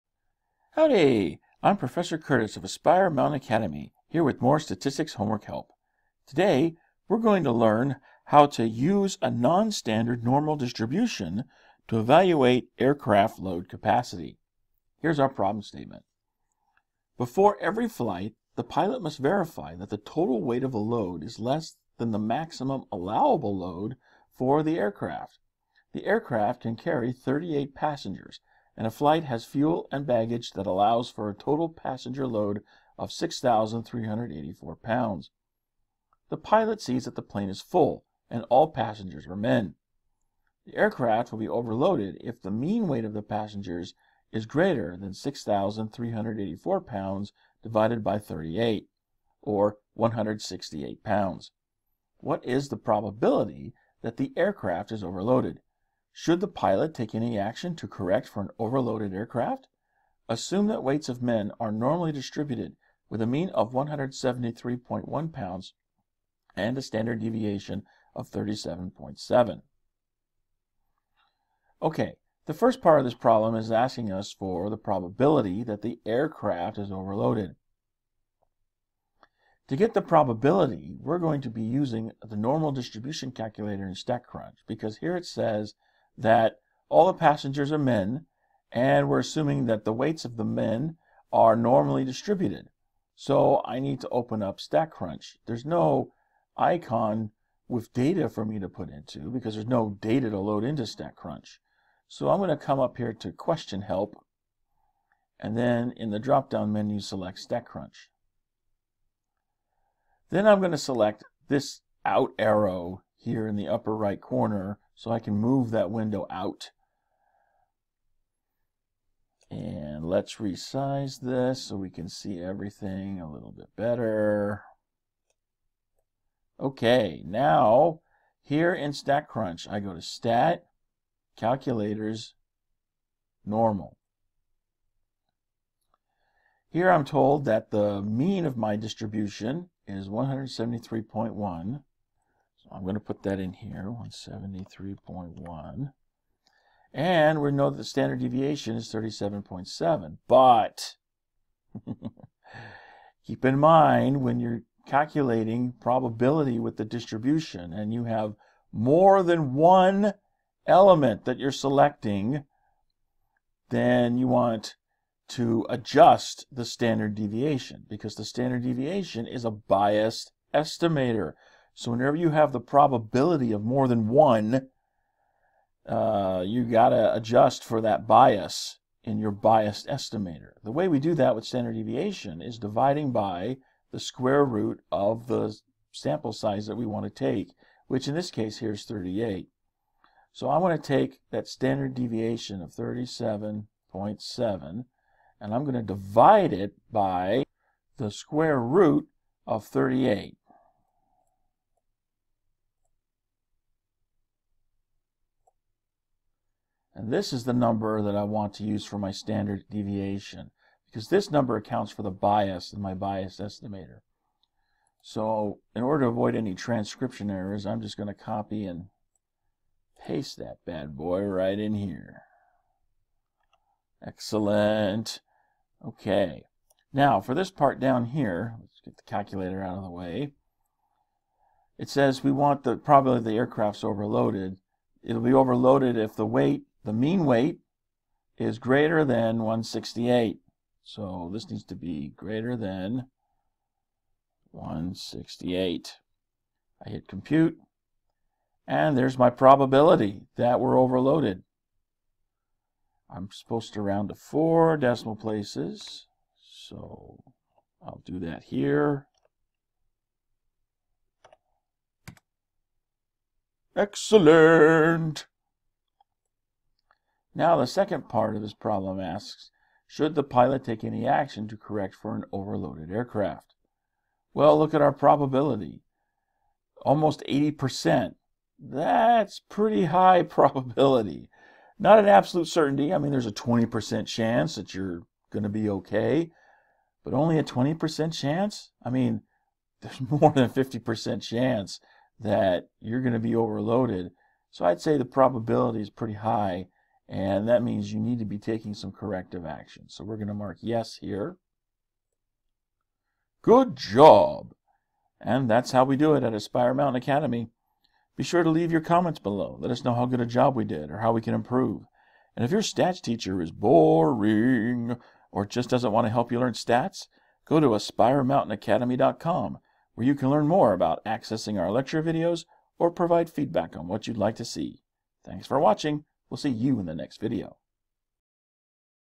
[6.4.20-T] Using a nonstandard normal distribution to evaluate aircraft load capacity
In this video, Professor Curtis uses StatCrunch to demonstrate how to use a nonstandard normal distribution to evaluate aircraft load capacity (MyStatLab ID# 6.4.20-T).